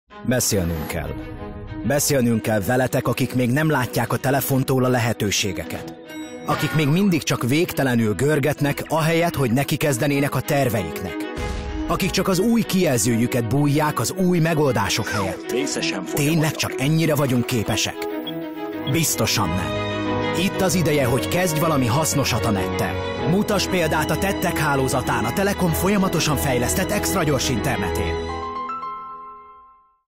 Mutass példát a Tettek Hálózatán!
Sültgalambra várás, halogatás, mások okolása, kifogások keresése, passzivitás... Tipikusan magyar dolgok, amiket meg kell változtatnunk! Kezdd el személyes előrejutásod érdekében használni az internetet! Olvass, tanulj, blogolj vagy vlogolj, indíts vállalkozást! A Telekom a Tettek Emberének építi és fejleszti folyamatosan az extragyors internetet nyújtó Tettek Hálózatát, ami otthon és a mobileszközeiden is elérhető. Legyen a Tettek Embere az új tipikusan magyar!